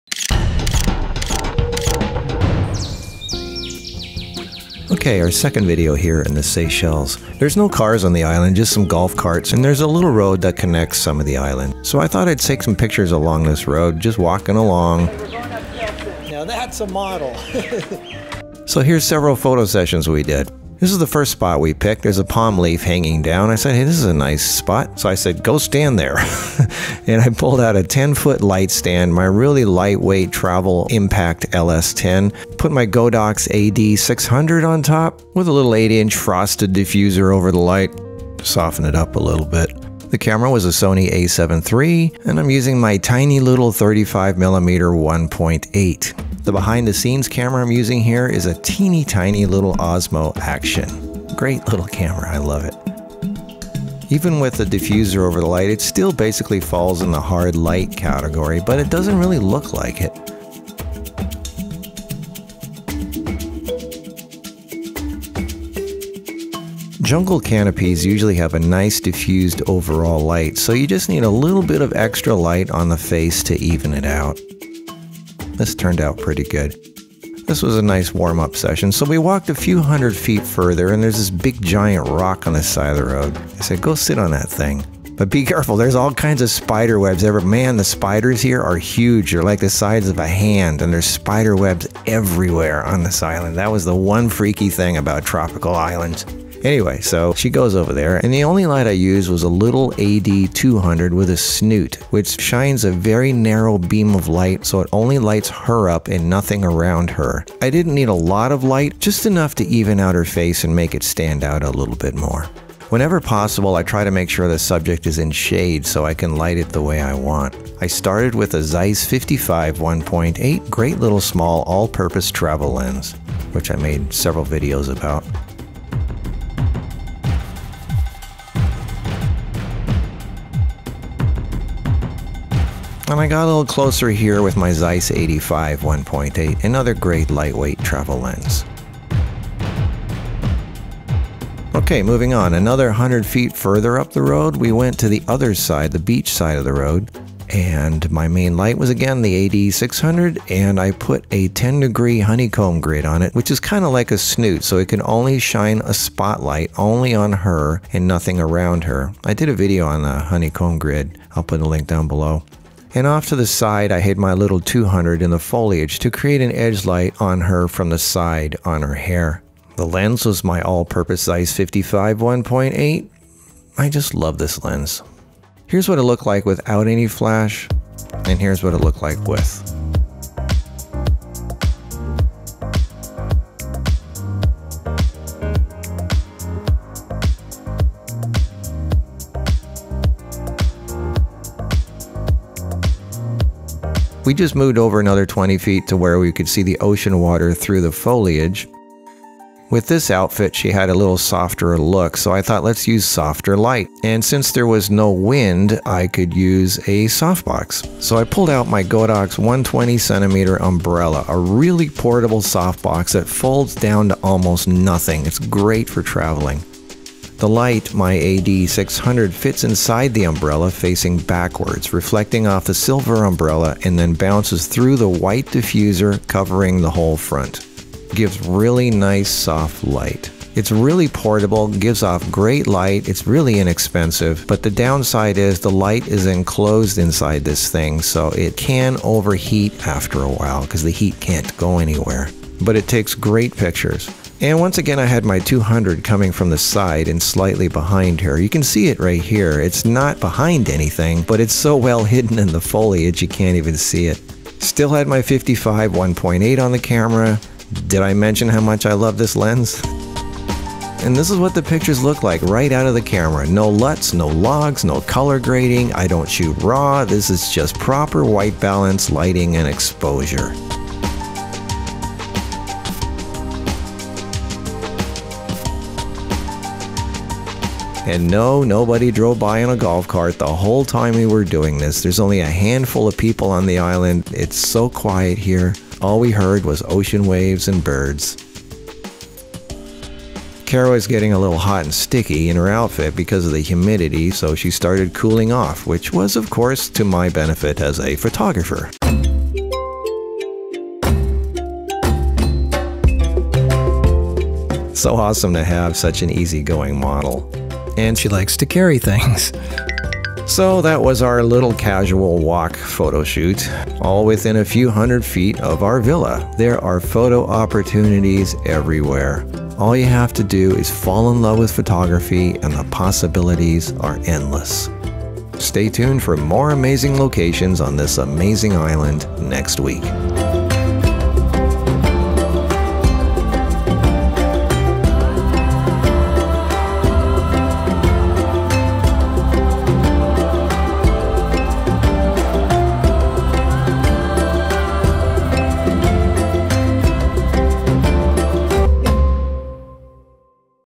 Four Different Simple Modifiers for Strobe Flash
A hands-on photography walkalong session on a jungle road on a tropical Island in the Seychelles with Markus and Cara. What could be a better way to spend a few minutes? Lots of different lighting discussed. Enjoy, be educated and inspired. Equipment used in this video is a Sony A7III full frame camera,
Sony 35mm f1.8 lens
Sony Zeiss 55mm f1.8 lens
Sony Zeiss 85mm f1.8 lens
tiny Osmo Action 4K camera for behind the scenes
Godox 120cm umbrella softbox
Godox AD600 flash strobe
Godox AD200 flash strobe
Godox X1 flash trigger
Godox honeycomb grid
Godox AD-S9 snoot for AD200